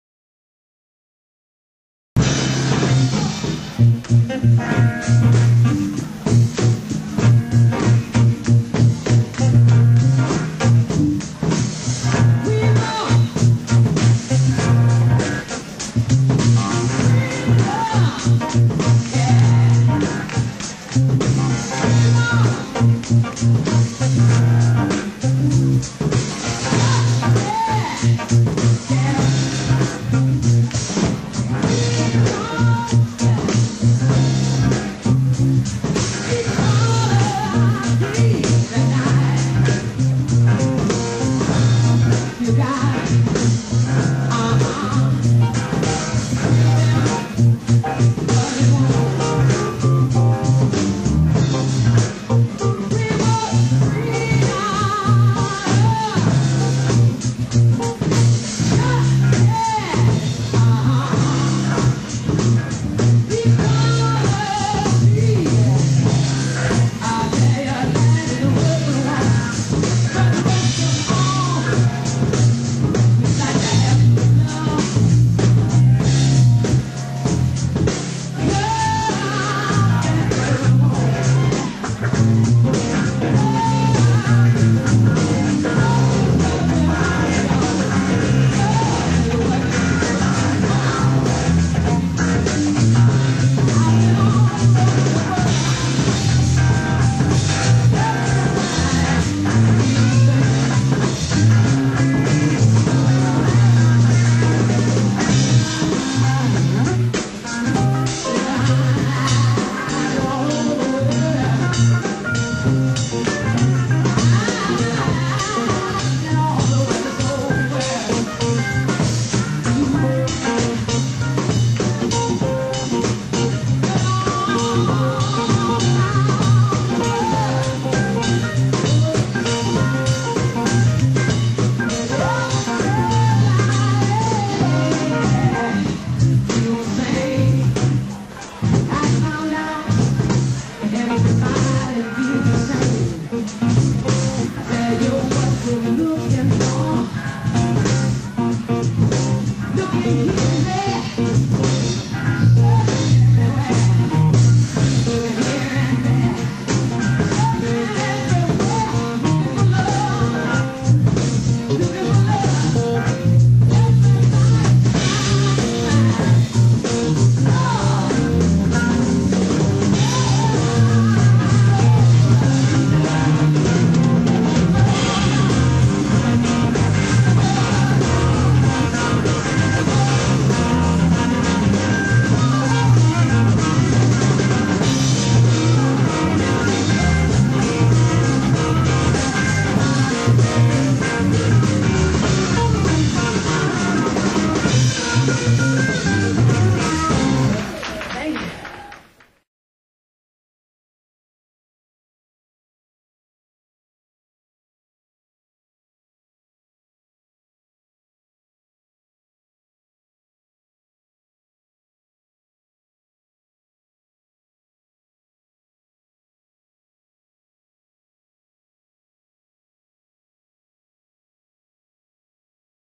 Thelma Jones Live 1976
Thelma Jones recorded at the Embers Club, Raleigh, North Carolina on May 3, 1976.